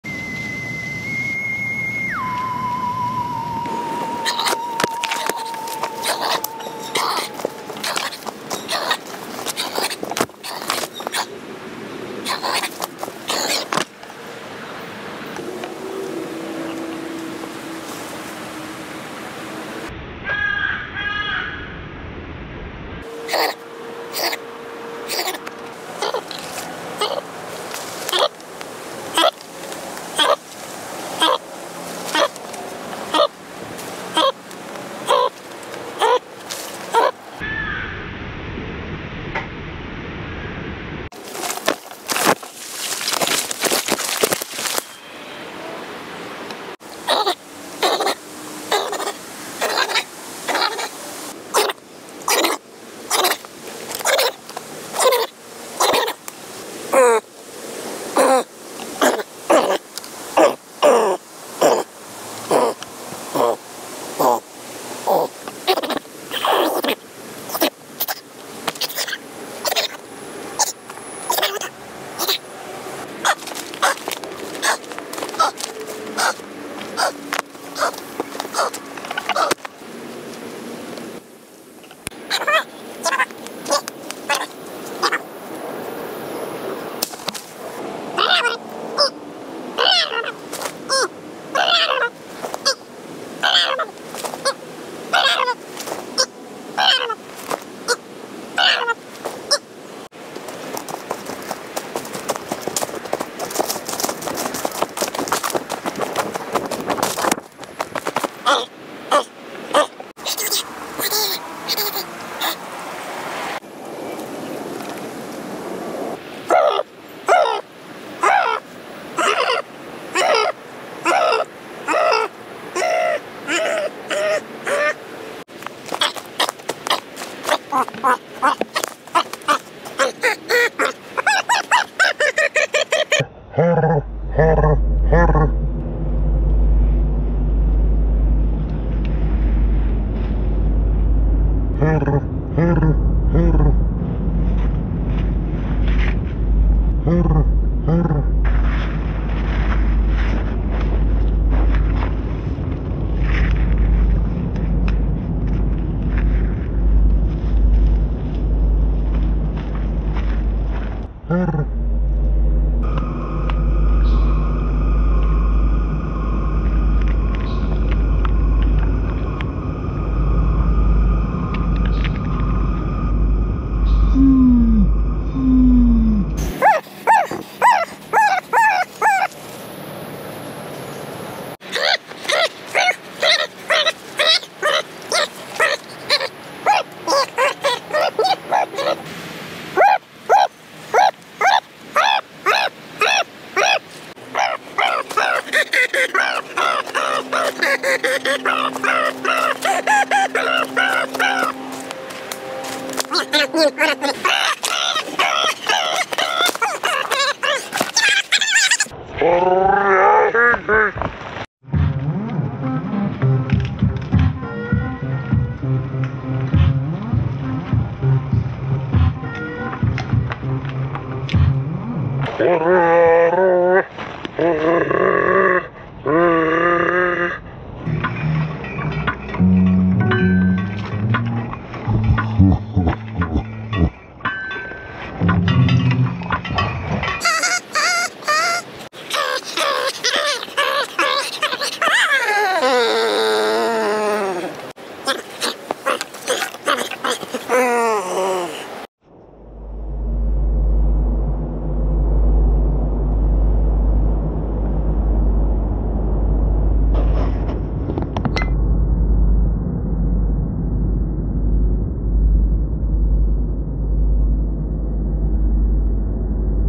Raccoon Shrooms: A Bad Trip
A sweet and innocent raccoon gets into some bad mushrooms. She starts to hallucinate and experience intense emotions. She thinks she's seeing a giant squirrel in an abandoned car.  She experiences vivid memories of fun interactions with a friendly human.  Are any of these memories real?  They are too specific to be fake, yet it's so over the top.  A raccoon wrestling a giant sloth?  A raccoon playing the piano?  These shrooms has her freaking out.
Join this raccoon, on its level, and experience the feeling of being both high and a raccoon at the same time.  The video is reminiscent of a David Lynch film.  Mysterious and fascinating, you’ll need to watch it again and again.